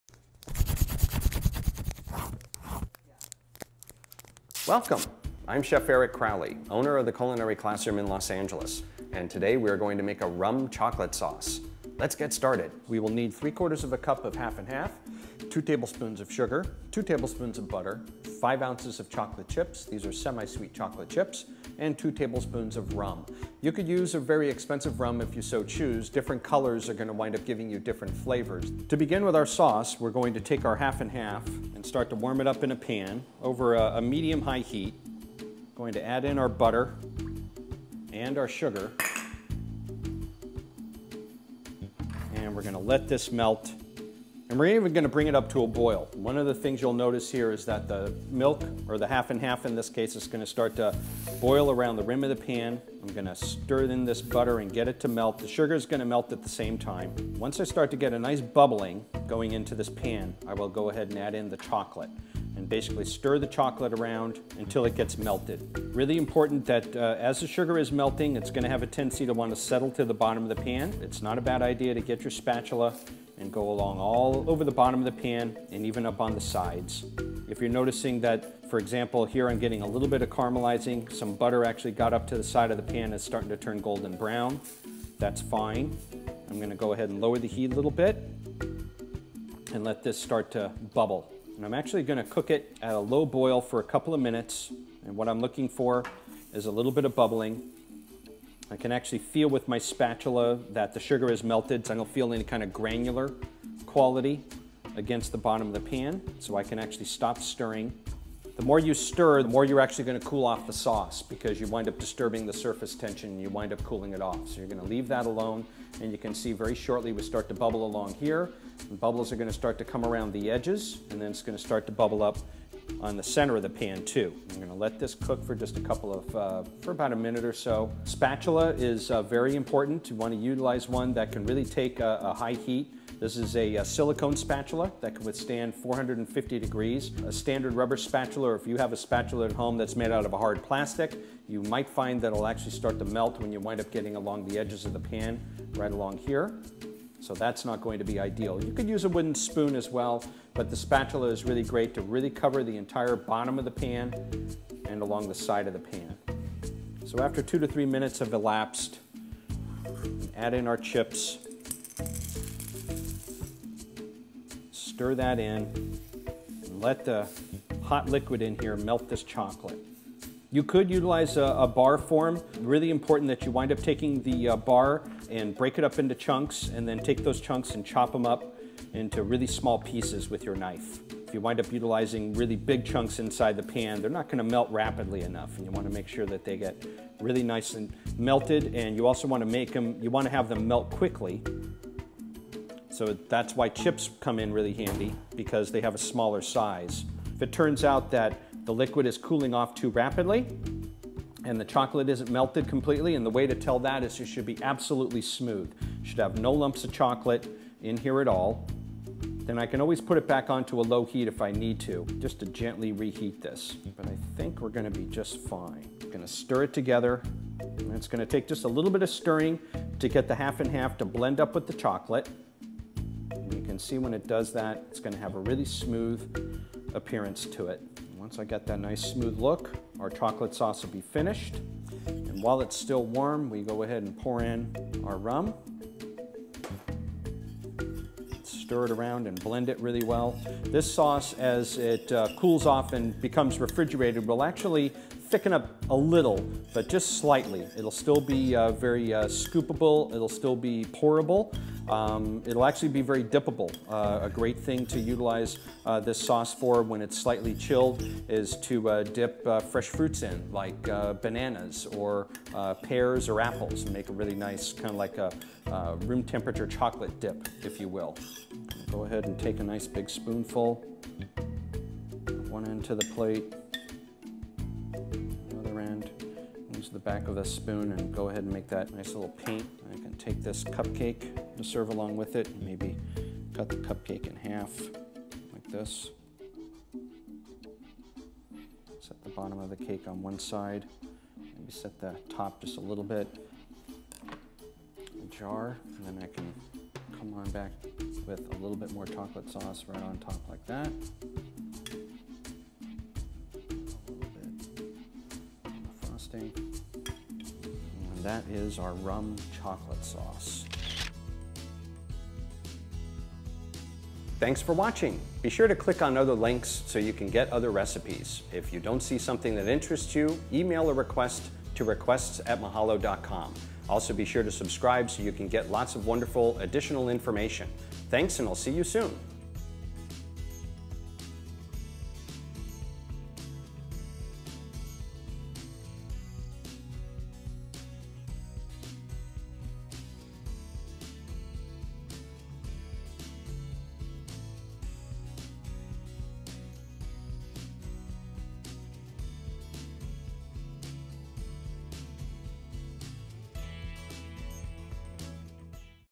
How to Make Rum Chocolate Sauce
In this video, Chef Eric Crowley, owner of the Culinary Classroom in Los Angeles, (‪ shows us how to make Rum Chocolate Sauce. You can use Rum Chocolate Sauce for frosting, drizzling, filling your favorite sweets and pastries! Yum!